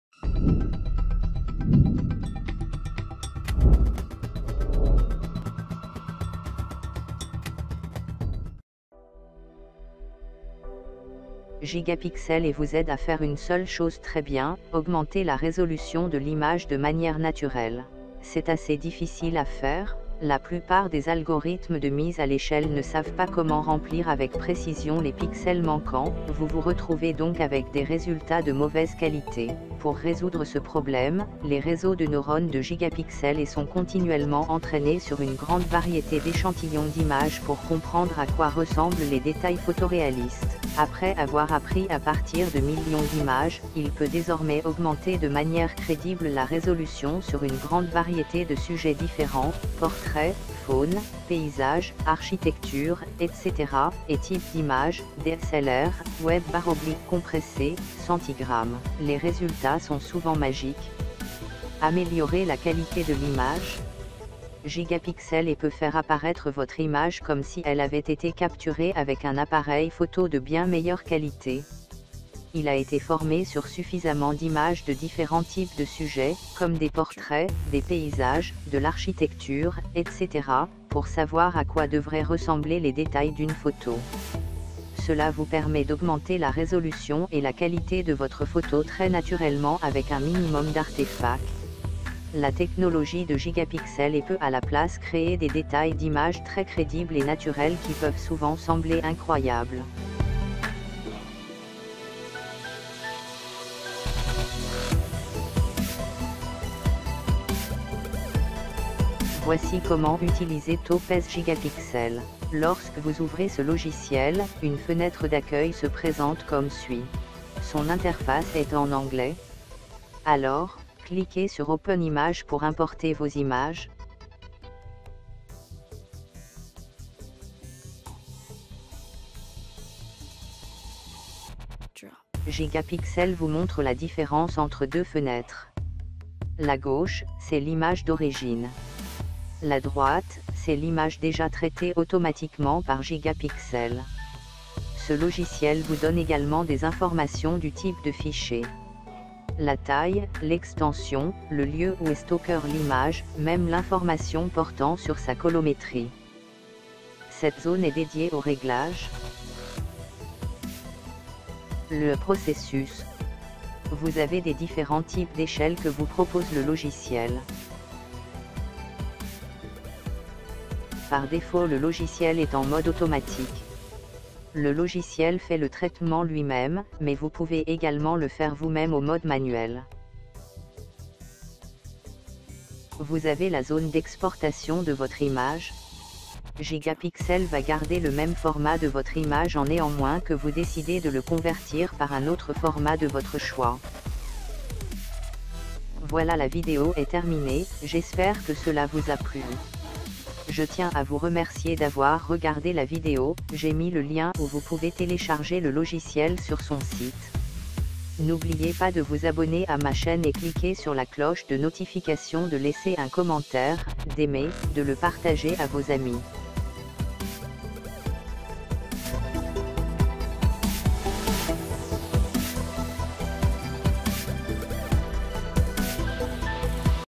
Tutoriel Gigapixel AI . French . TJP FX
Gigapixel AI vous aide à faire une seule chose très bien : augmenter la résolution de l'image de manière naturelle. C'est assez difficile à faire. La plupart des algorithmes de mise à l'échelle ne savent pas comment remplir avec précision les pixels manquants, vous vous retrouvez donc avec des résultats de mauvaise qualité.